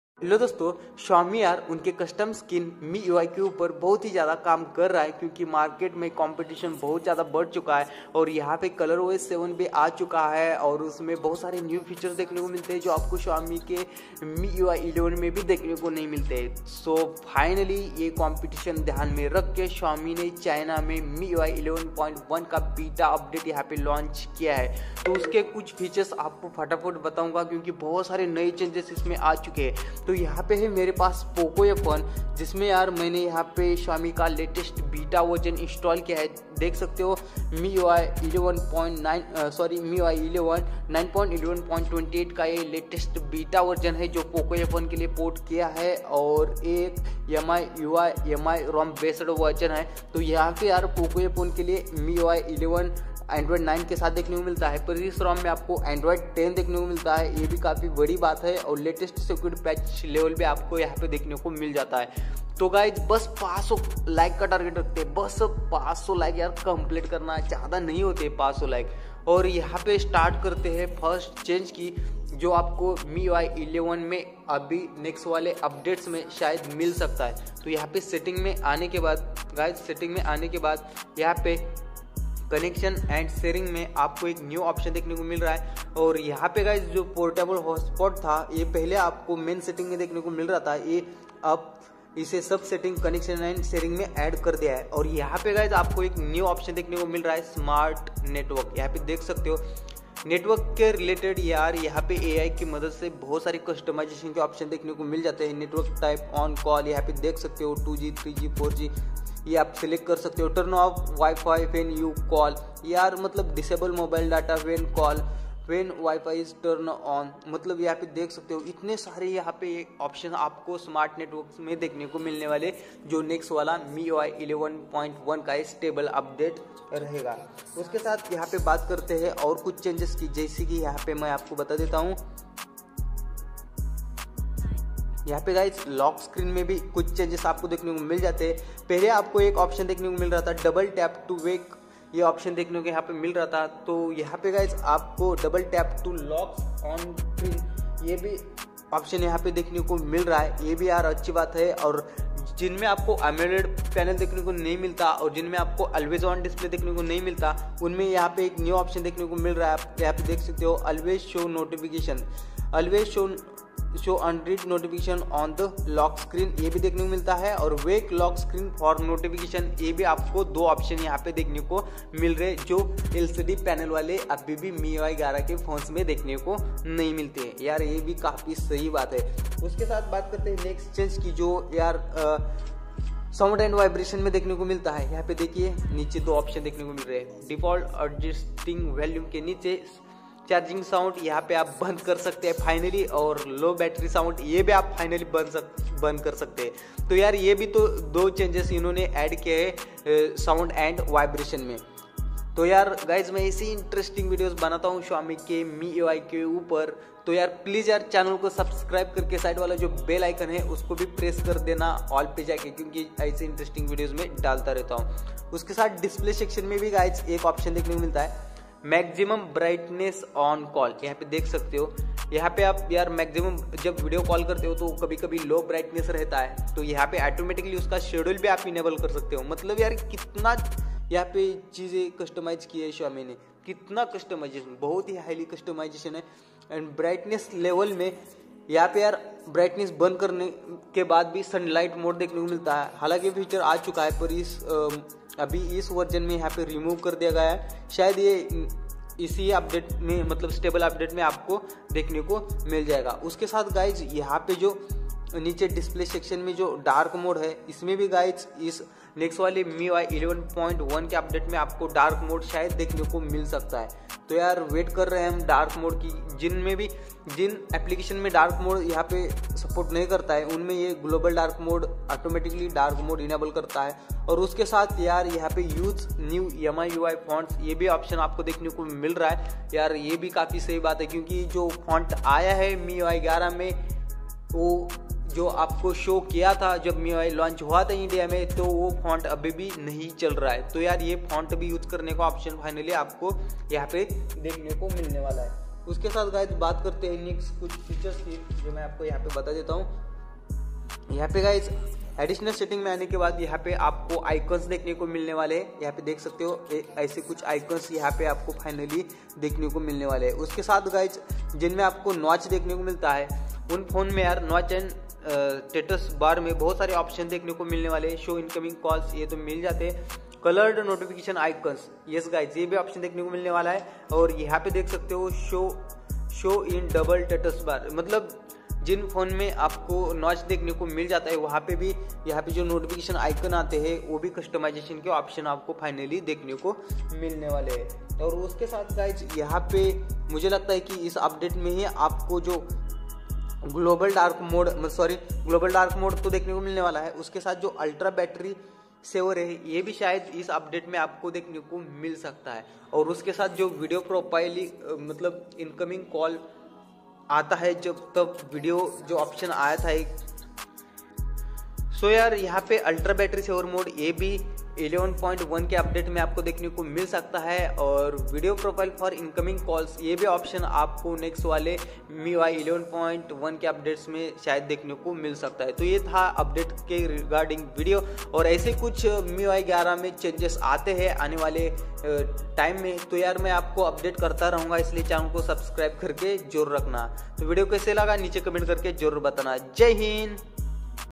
MIUI 11.1 (10+ ) New Features | Smart Mobile network
Guy my name is madan Gaikwad and I'm show on this video MIUI 11.1 (10+ ) New Features | Smart Mobile network.